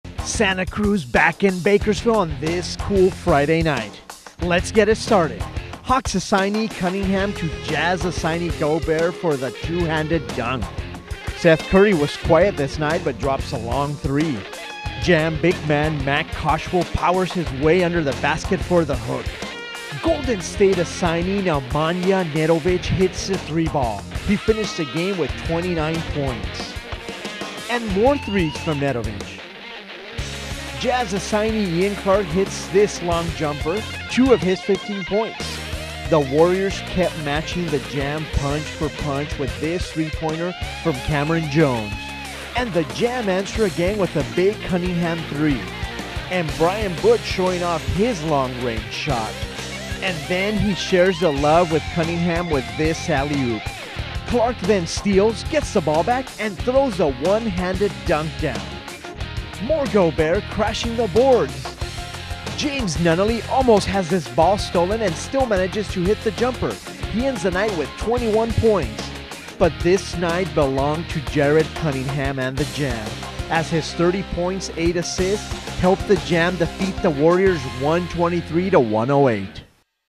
Highlights: Hawks assignee Jared Cunningham drops 30 to lead Jam past Nedovic & Santa Cruz
Bakersfield Jam 123  Santa Cruz Warriors 108    [From 12/20/13]

The Bakersfield Jam defeat the Santa Cruz Warriors behind Atlanta Hawks asignee Jared Cunningham's 30 points. Warriors rookie Nemanja Nedovic returned on assignment to Santa Cruz, pouring in 29 points of his own despite the loss.